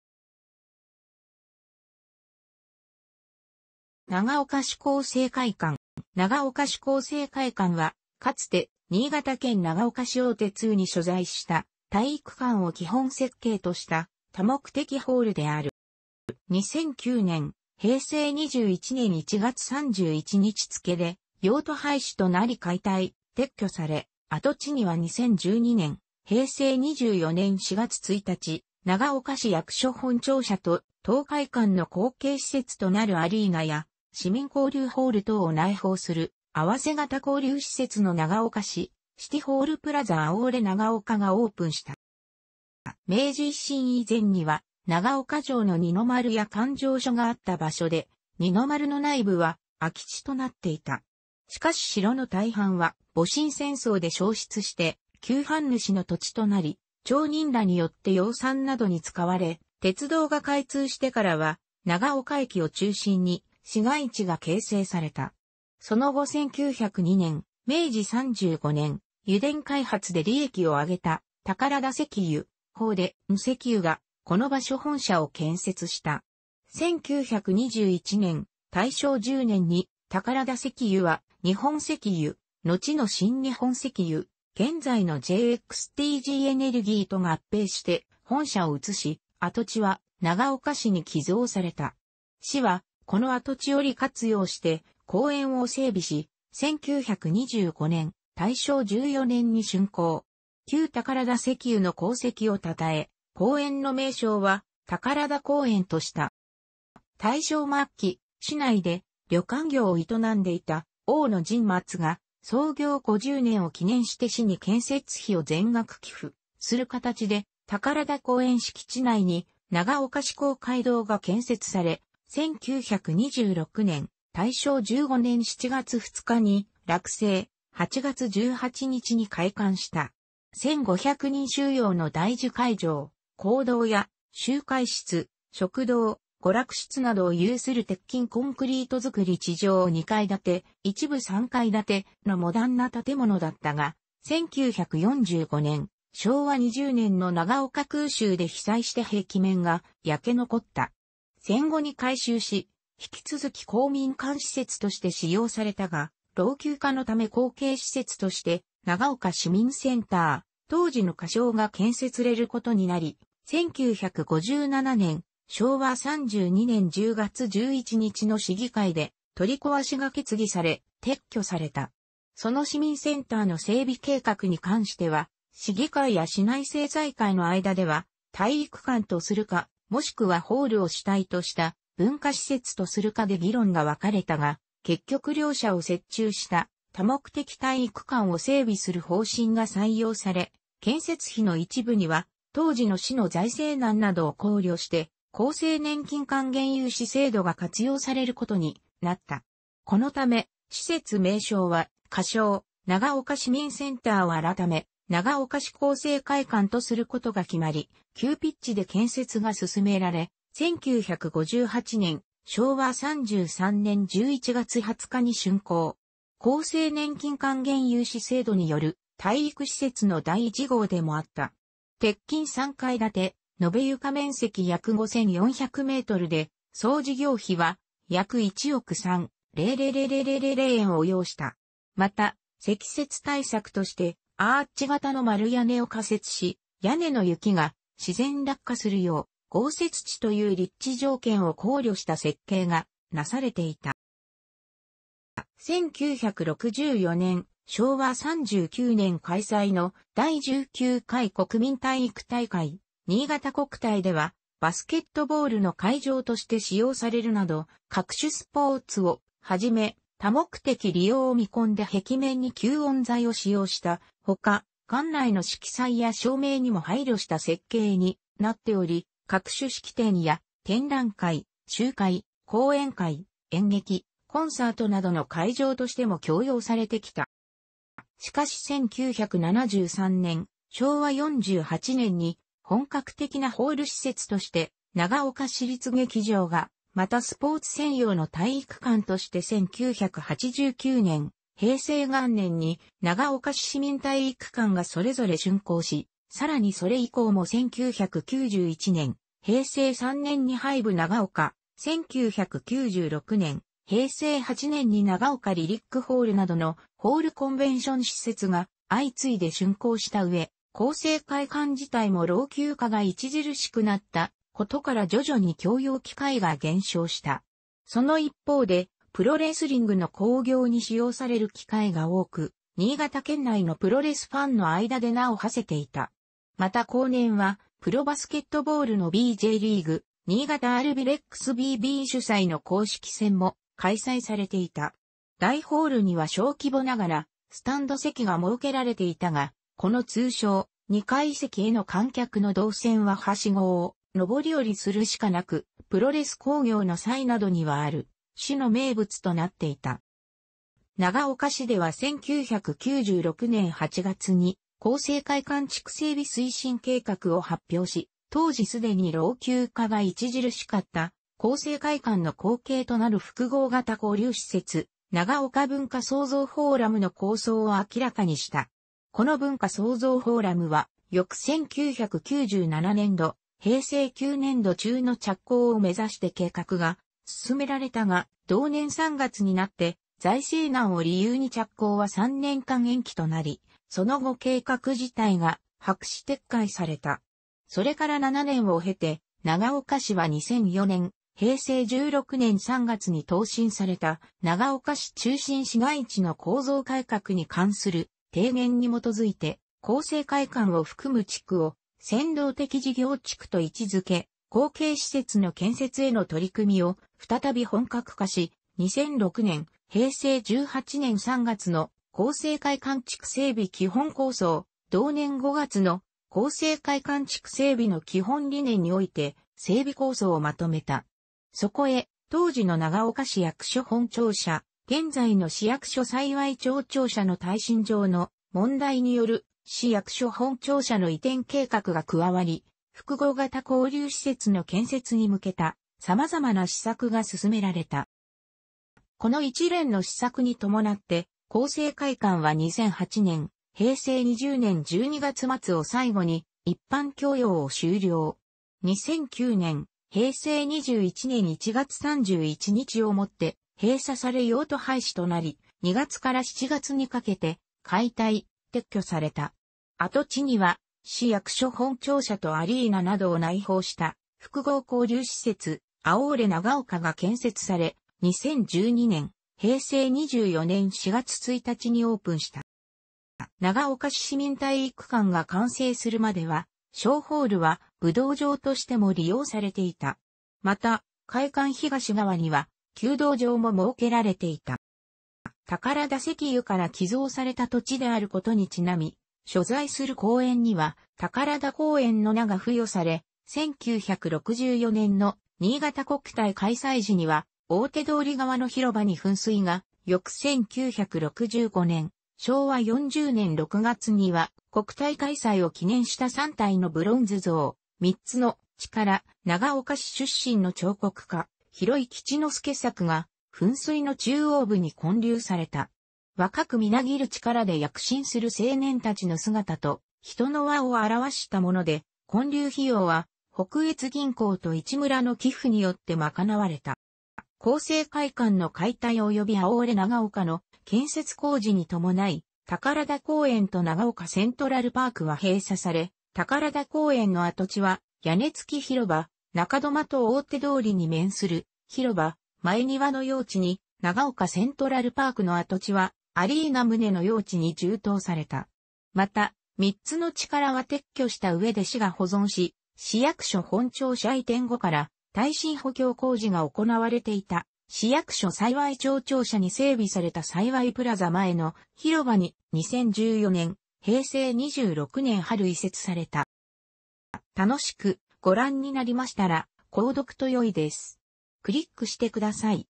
長岡市厚生会館
長岡市厚生会館（ながおかしこうせいかいかん）は、かつて新潟県長岡市大手通に所在した、体育館を基本設計とした多目的ホールである。

2009年（平成21年）1月31日付で用途廃止となり解体・撤去され、跡地には2012年（平成24年）4月1日、長岡市役所本庁舎と、当会館の後継施設となるアリーナや市民交流ホール等を内包する複合型交流施設の長岡市シティホールプラザ アオーレ長岡がオープンした。

明治維新以前には長岡城の二の丸や勘定所があった場所で、二の丸の内部は空き地となっていた。しかし城の大半は戊辰戦争で焼失して旧藩主の土地となり、町人らによって養蚕などに使われ、鉄道が開通してからは長岡駅を中心に市街地が形成された。その後1902年（明治35年）、油田開発で利益を上げた「宝田石油」（ほうでんせきゆ）がこの場所に本社を建設した。1921年（大正10年）に宝田石油は日本石油（のちの新日本石油、現在のJXTGエネルギー）と合併して本社を移し、跡地は長岡市に寄贈された。市はこの跡地を利活用して公園を整備し、1925年（大正14年）に竣工。旧宝田石油の功績を讃え、公園の名称は「宝田公園」とした。
大正末期、市内で旅館業を営んでいた大野甚松が、創業50年を記念して市に建設費を全額寄付する形で、宝田公園敷地内に長岡市公会堂が建設され、1926年（大正15年）7月2日に落成、8月18日に開館した。1500人収容の大集会場（講堂）や集会室、食堂、娯楽室などを有する鉄筋コンクリート造地上2階建（一部3階建）のモダンな建物だったが、1945年（昭和20年）の長岡空襲で被災して壁面が焼け残った。戦後に改修し、引き続き公民館施設として使用されたが、老朽化のため後継施設として「長岡市民センター」（当時の仮称）が建設されることになり、1957年（昭和32年）10月11日の市議会で取り壊しが決議され、撤去された。
その市民センターの整備計画に関しては、市議会や市内政財界の間では体育館とするか、もしくはホールを主体とした文化施設とするかで議論が分かれたが、結局両者を折衷した多目的体育館を整備する方針が採用され、建設費の一部には当時の市の財政難などを考慮して厚生年金還元融資制度が活用されることになった。このため施設名称は仮称「長岡市民センター」を改め「長岡市厚生会館」とすることが決まり、急ピッチで建設が進められ、1958年（昭和33年）11月20日に竣工。厚生年金還元融資制度による体育施設の第1号でもあった。鉄筋3階建、延床面積約5,400mで、総事業費は約1億3,000万円を要した。また積雪対策としてアーチ型の丸屋根を架設し、屋根の雪が自然落下するよう、豪雪地という立地条件を考慮した設計がなされていた。

1964年（昭和39年）開催の第19回国民体育大会（新潟国体）ではバスケットボールの会場として使用されるなど各種スポーツをはじめ、多目的利用を見込んで壁面に吸音材を使用した他、館内の色彩や照明にも配慮した設計になっており、各種式典や展覧会、集会、講演会、演劇、コンサートなどの会場としても供用されてきた。

しかし1973年（昭和48年）に本格的なホール施設として長岡市立劇場が、またスポーツ専用の体育館として1989年（平成元年）に長岡市市民体育館がそれぞれ竣工し、さらにそれ以降も1991年（平成3年）にハイブ長岡、1996年（平成8年）に長岡リリックホールなどのホール・コンベンション施設が相次いで竣工した上、厚生会館自体も老朽化が著しくなったことから徐々に供用機会が減少した。その一方でプロレスリングの興行に使用される機会が多く、新潟県内のプロレスファンの間で名を馳せていた。また後年はプロバスケットボールのbjリーグ・新潟アルビレックスBB主催の公式戦も開催されていた。大ホールには小規模ながらスタンド席が設けられていたが、この通称「2階席」への観客の動線ははしごを上り下りするしかなく、プロレス興行の際などにはある種の名物となっていた。

長岡市では1996年8月に「厚生会館地区整備推進計画」を発表し、当時既に老朽化が著しかった厚生会館の後継となる複合型交流施設「長岡文化創造フォーラム」の構想を明らかにした。この文化創造フォーラムは翌...